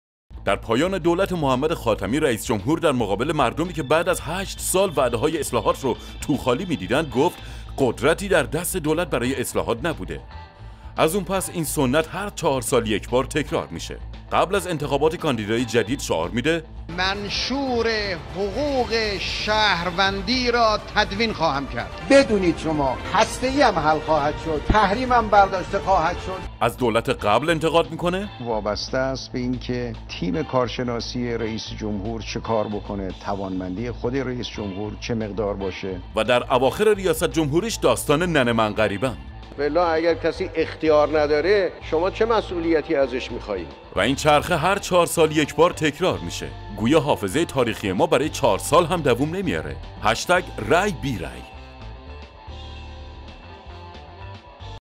انتخابات فرمالیته
ملت ما رو سرگرم کردن با انتخابات که آینده تون در گرو انتخاب امروزه. اما حالا دیگه فهمیدیم تو این چهل سال. و‌حالا تصمیم گرفتیم دیگه رأی_بی_رأی. موافقید؟